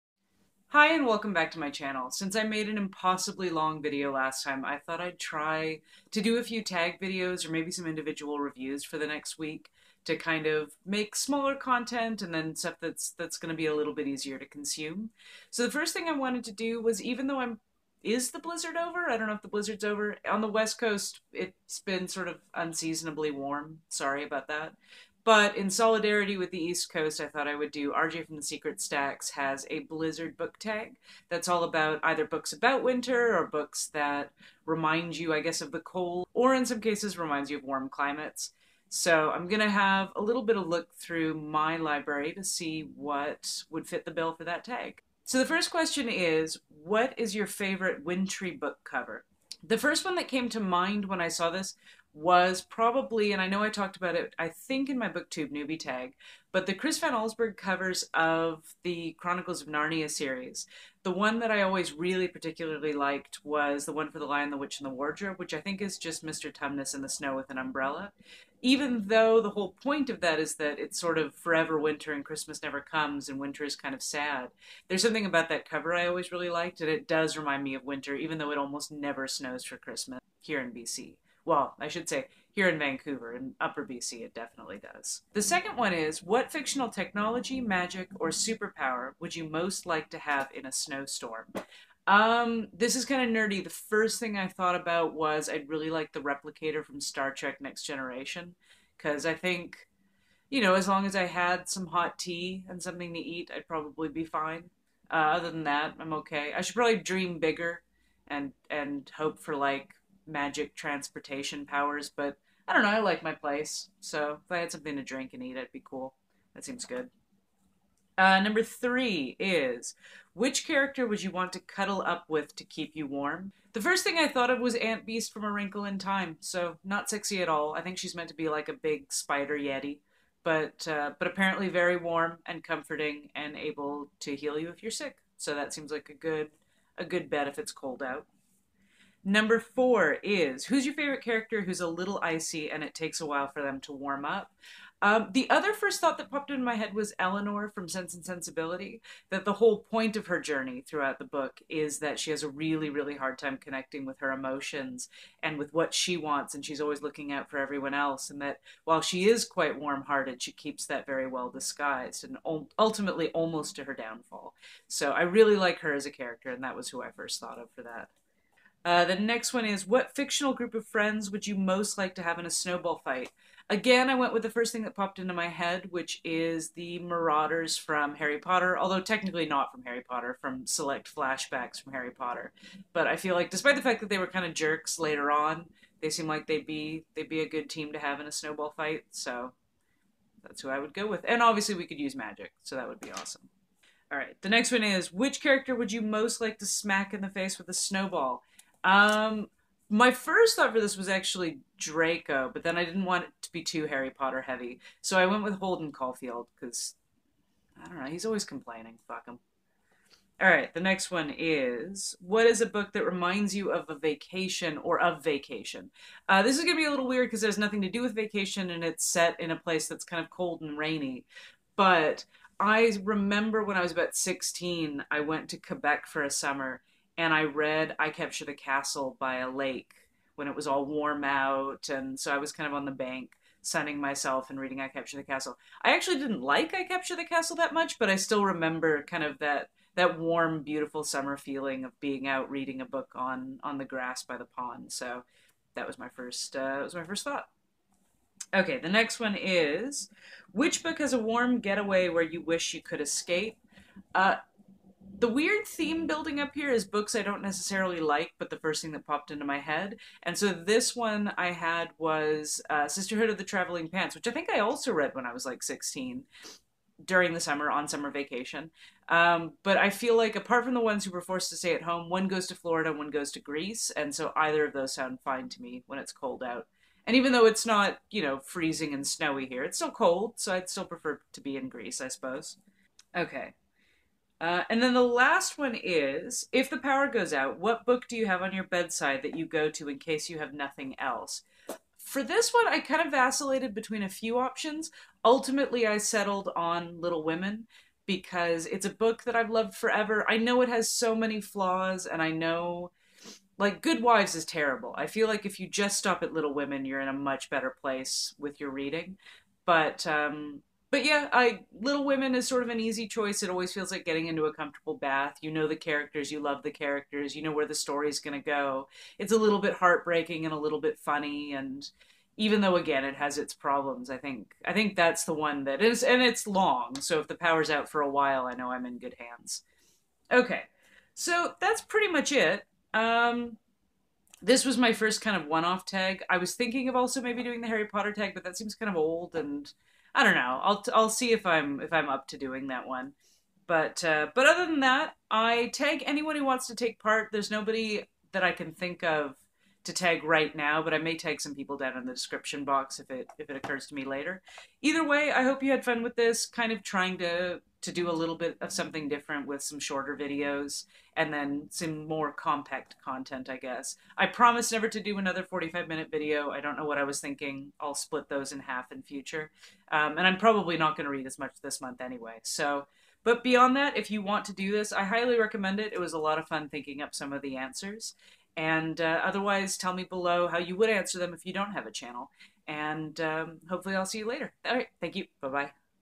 Blizzard Book Tag | Wintery Book Recommendations
I did RJ's Blizzard Book Tag.

I tag anyone who wants to take part, and here are the questions:

1. What is your favorite wintry book cover?

2. What fictional technology, magic, or superpower, would you most like to have in a snowstorm?

3. Which character would you want to cuddle up with to keep you warm?

4. Who's your favorite character who's a bit icy and it takes a little while for them to warm up?

5. What fictional group of friends would you want on your team for a snowball fight?

6. What character would you want to smack in the face with a snowball?

7. A book that reminds you of vacation.

8. What book has warm getaway where you wish you could escape?

9. Power goes out. What book do you have on your bedside or loaded on your kindle or iPad that's your go to in case you don't have anything else?

Books mentioned in this video:

1) The Lion, the Witch, and the Wardrobe -- C.S. Lewis

2) A Wrinkle in Time -- Madeleine L'Engle

3) Sense and Sensibility -- Jane Austen

4) Harry Potter and the Order of the Phoenix -- JK Rowling

5) Catcher in the Rye -- JD Salinger

6) I Capture the Castle -- Dodie Smith

7) Little Women -- Louisa May Alcott

8) The Sisterhood of the Traveling Pants -- Ann Brashares